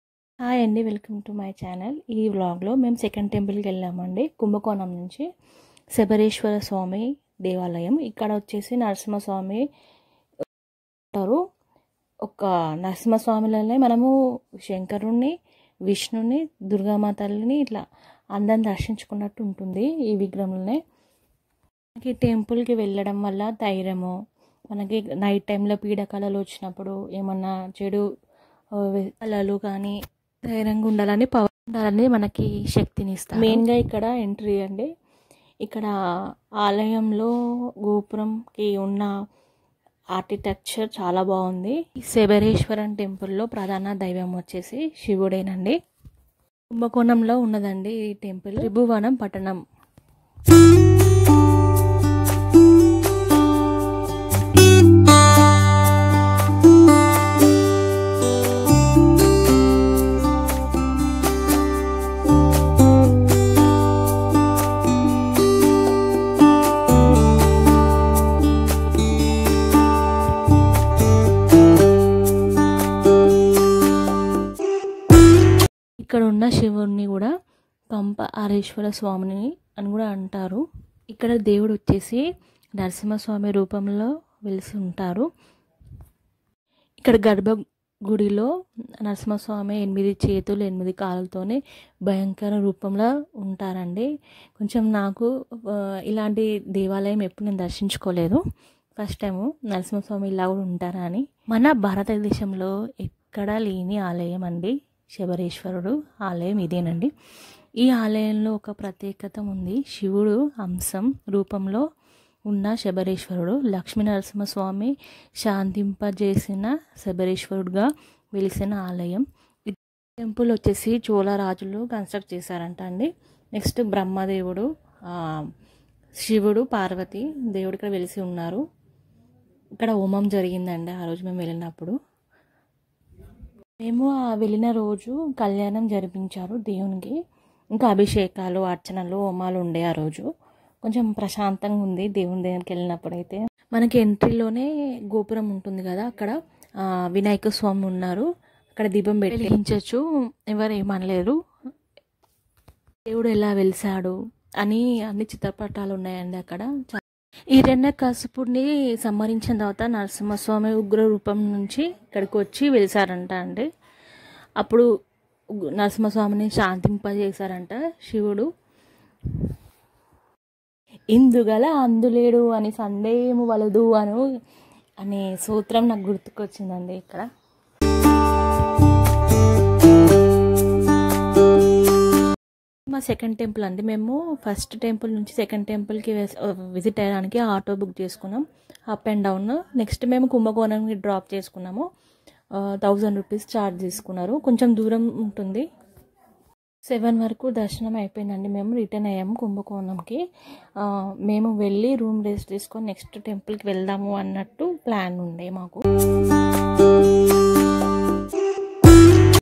Sri Sarabeswarar Temple full details in telugu|Sharabha is an eight -legged/part-lion and part-bird
Sarabeshwarar temple complete information in telugu and how to reach from Kumbakonam to Sarabeswarar temple details.

Sharabha is an eight -legged/part-lion and part-bird and also more powerful than a lion or an elephant.

Train: Secunderabad JN to MGR CHENNAI CTL(12604)
Departs: Secunderabad JN 17:10(Wednesday)
Arrives: MGR CHENNAI CTL 05:40(Thursday)